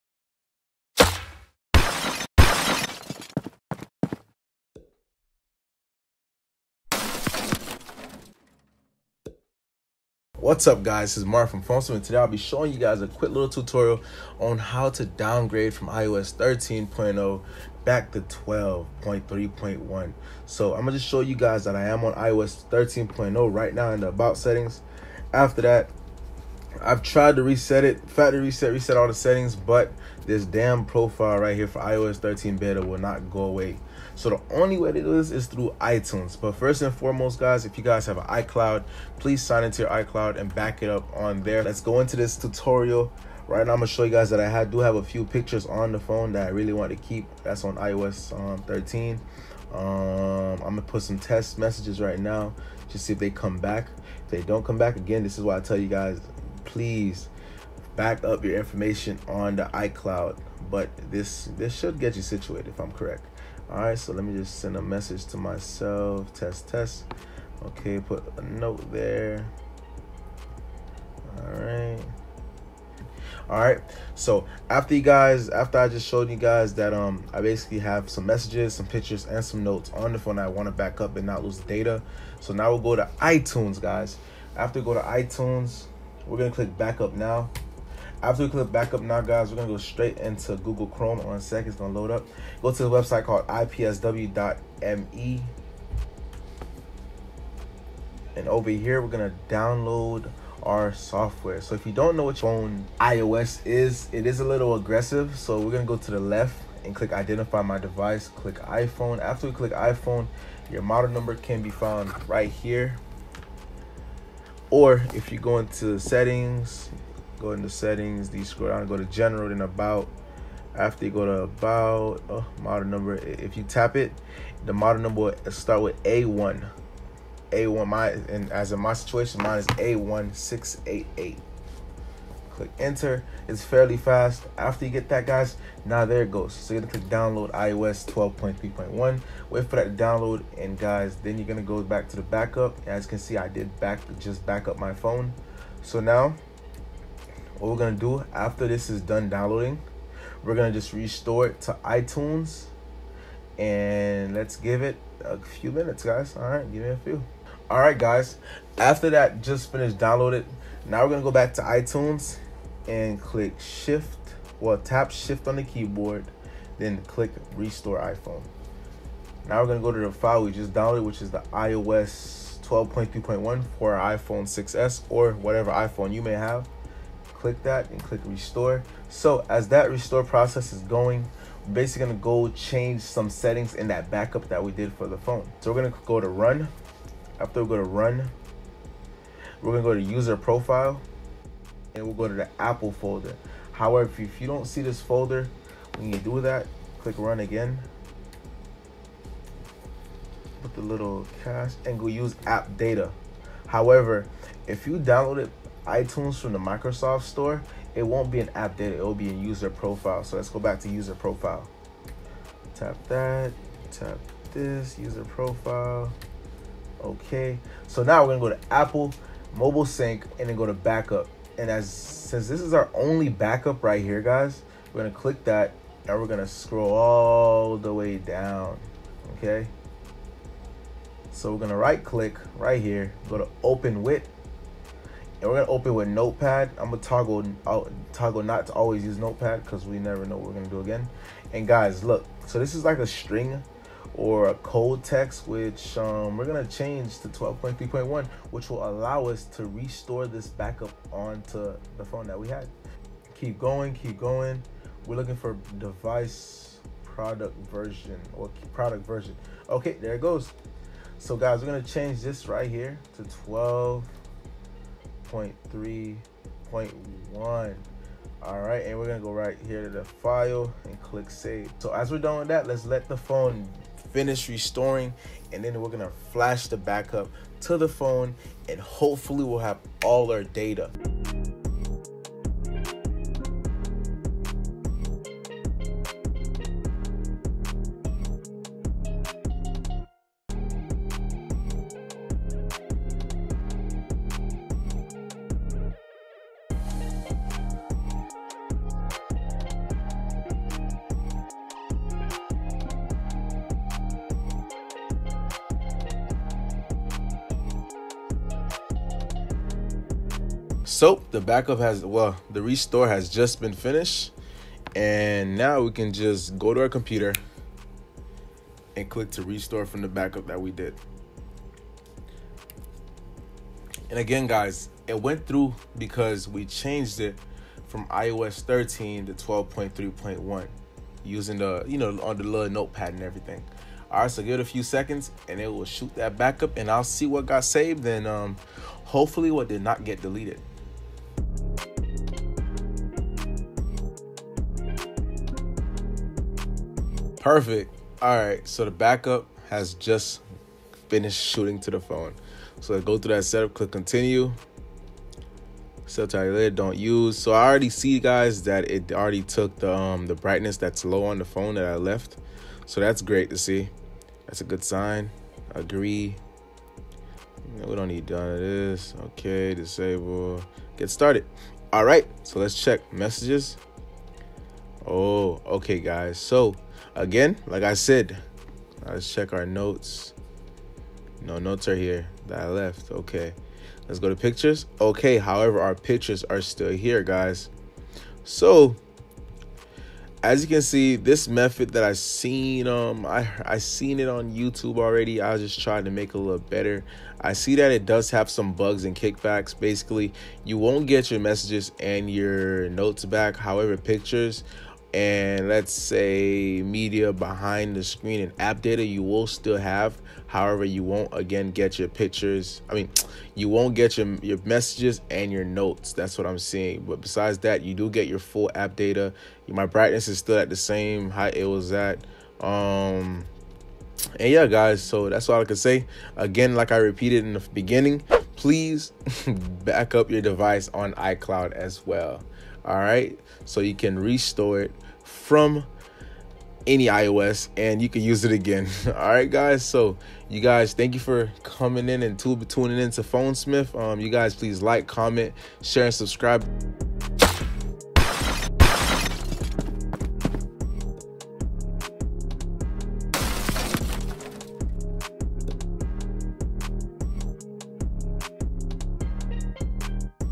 DOWNGRADE IOS 13 BETA 2 TO IOS 12.3.1 (NO DATA LOSS) ICLOUD BACKUP *JULY UPDATE*
Fliest way to keep all your data when leaving IOS 13 BETA 2!! Let's go back to 12.3.1 with no setback USING WINDOWS PC with the most UPDATED JULY METHOD!*BACK UP PHONE ON ICLOUD 2* I REPEATT!

LINKS: IPSW.ME (FOR IOS FIRMWARE)